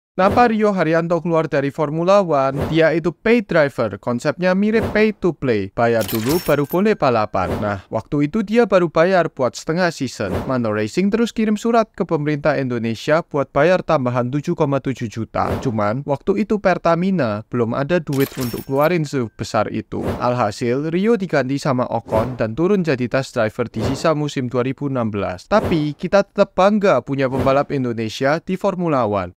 Kenapa Rio Haryanto Keluar dari F1?
Di video YouTube Shorts ini, kita akan bahas tentang Rio Haryanto, pembalap F1 pertama dari Indonesia.

Tapi, kariernnya di F1 sangatlah singkat, dia terpaksa harus meninggalkan F1 setelah hanya setengah musim (6 bulan).

Alasannya: kesulitan dana & untuk membayar kursi F1 (sekitar US$15 juta). Alhasil, Rio diganti sama Esteban Ocon.

Topik: Rio Haryanto, Formula 1, Pay Driver, Pertamina, Manor Racing, Ocon, Test Driver, Indonesia di F1, Sponsorship, Manor Racing Team.